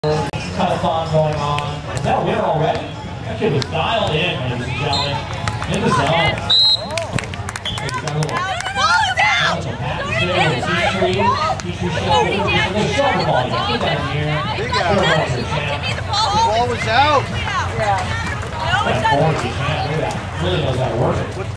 Elaine Youngs and Rachel Wacholder
Elaine Youngs gets heated on the sand after a blown call by the officials.